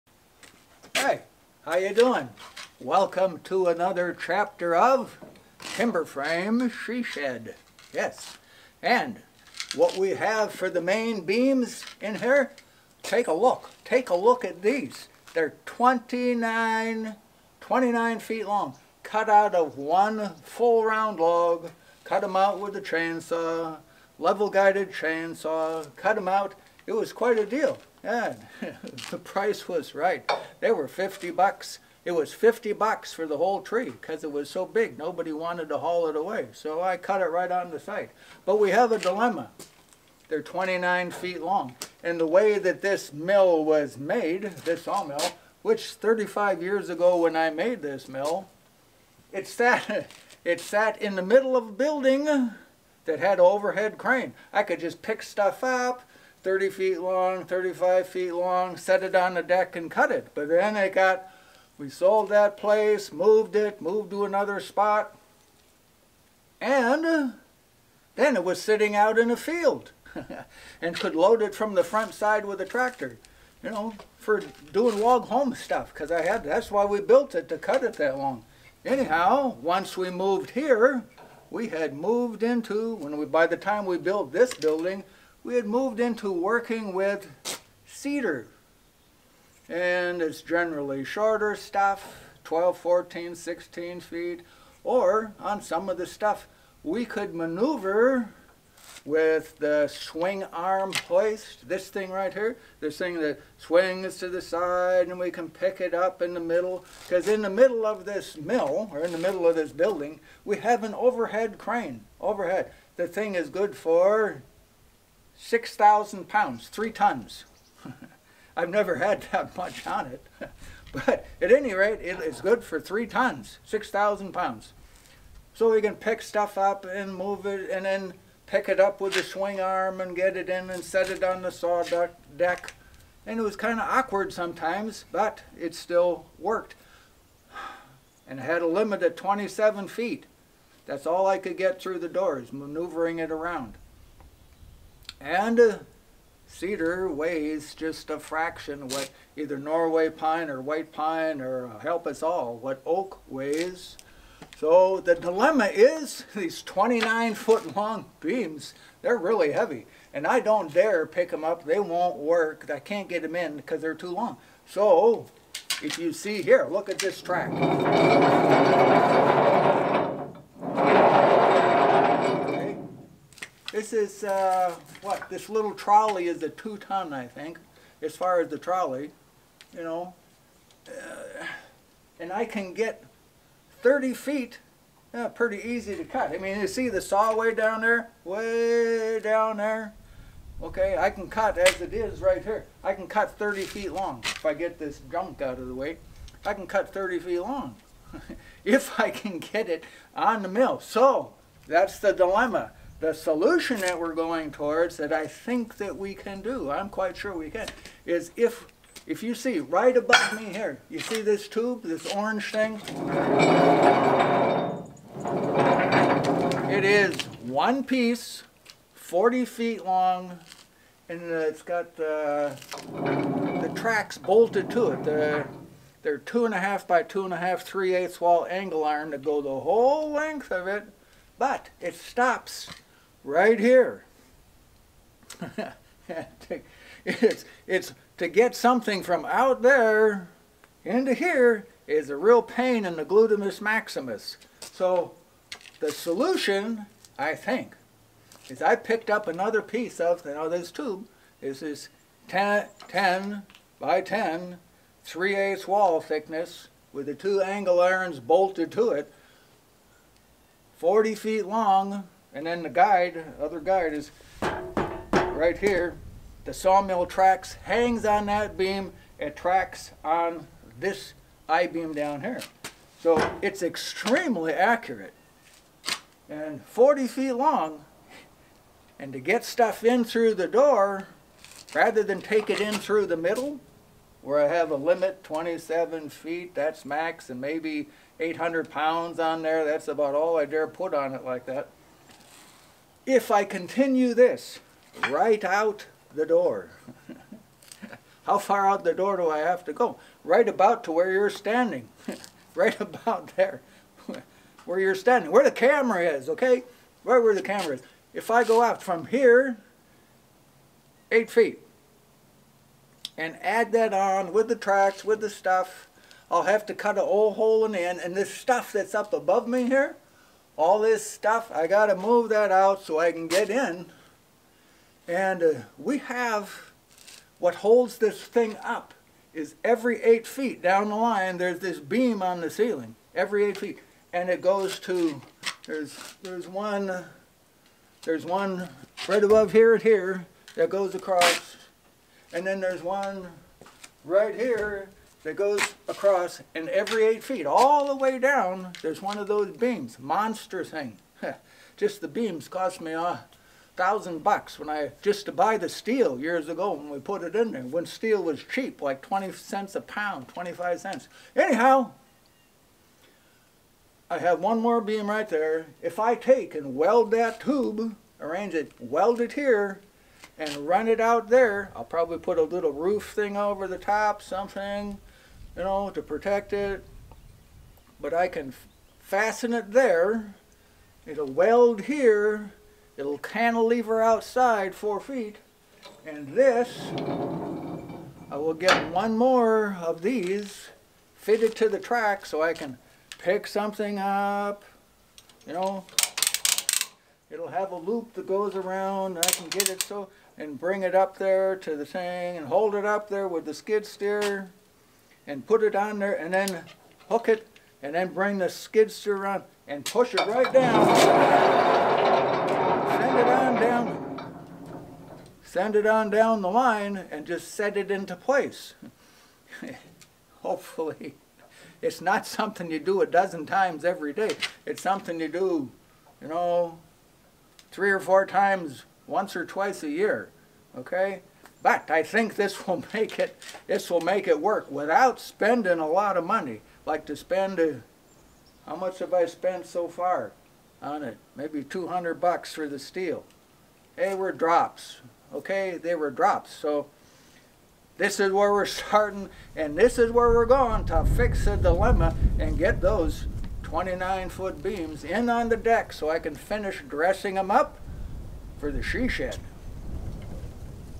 Timber Frame She Shed; #2 Sawmill Dilemma
Can't get 29 foot long material onto the saw deck; yet. See here how we use to accurately cut a monster log into four 29' long 12" x 12" timbers, with just a chalk line and a chainsaw fitted with a bubble level; and try to figure out how to change our home made sawmill to handle a 30 foot long monster log.